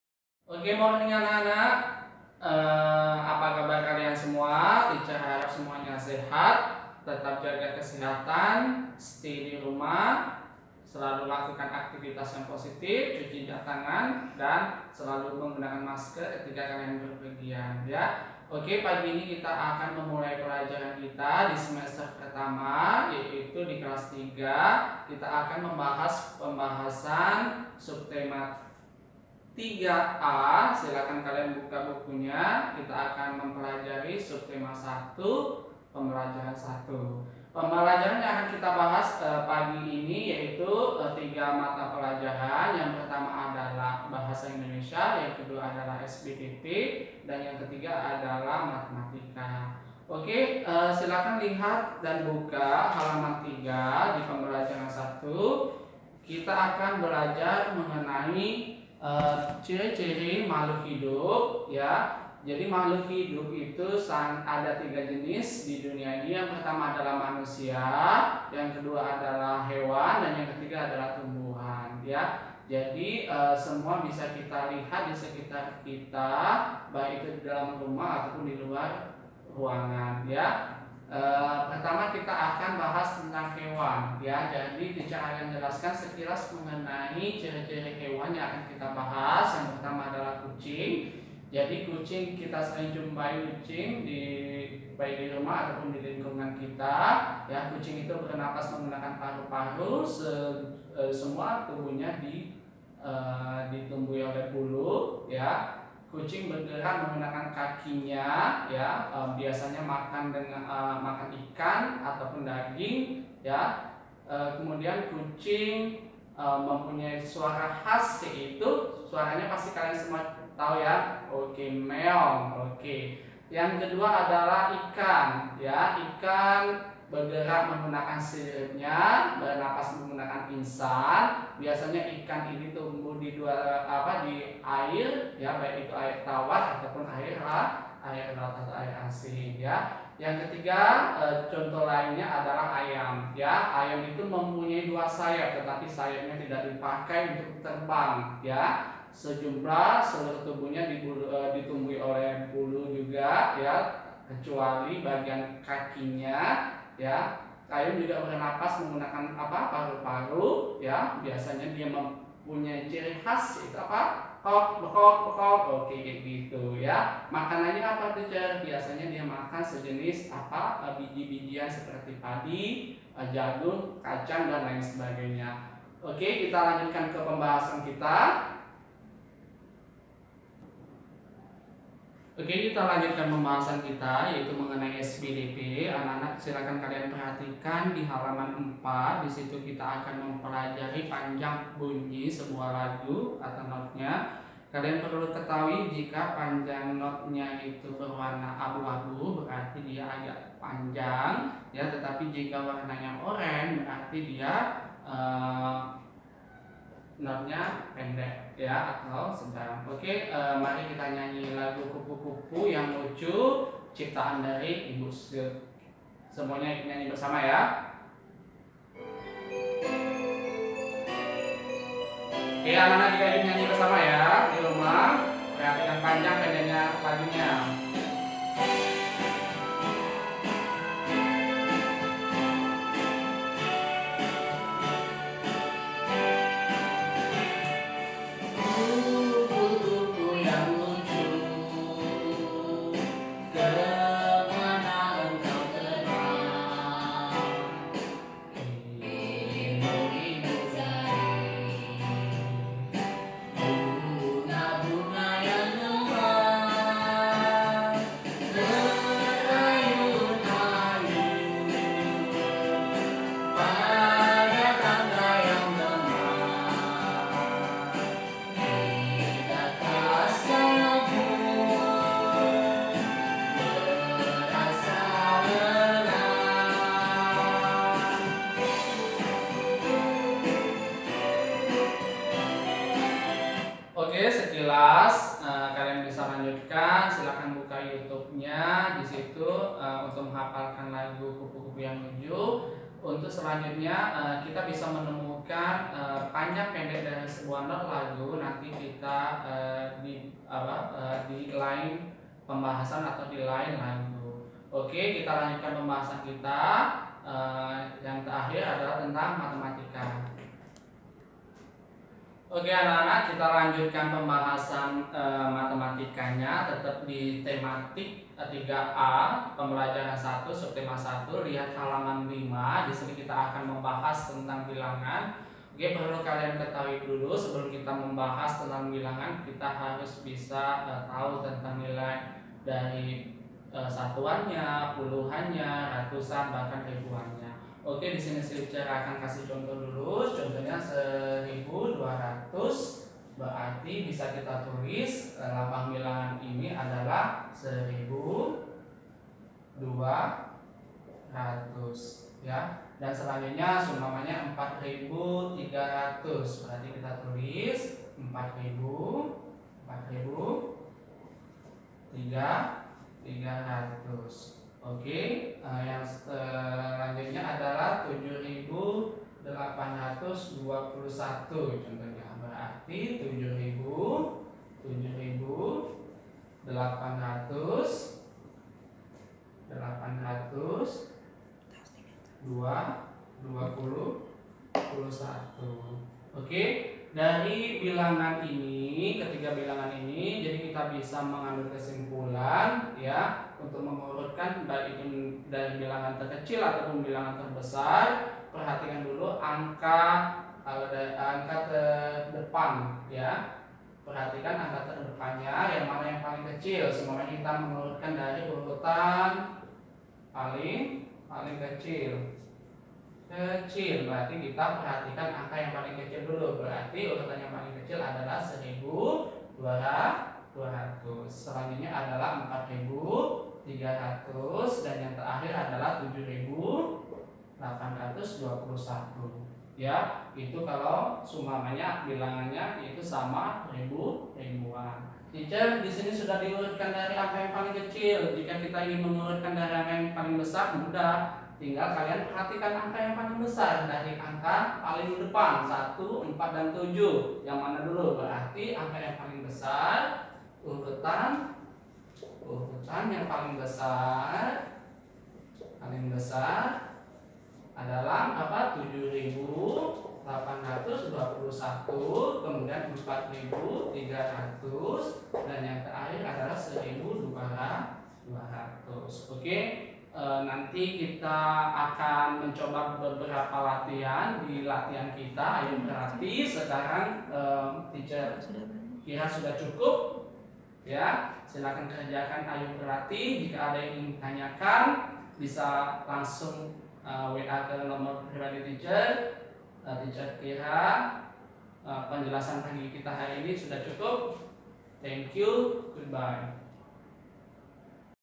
SD MANGGALA GRADE 3 Pembelajaran 1, 14 Juli 2020
Mikail Latif, S.Pd is usually called Teacher Mikail . He is Homeroom teacher of grade 3 (three). He teaches Tematik and Math.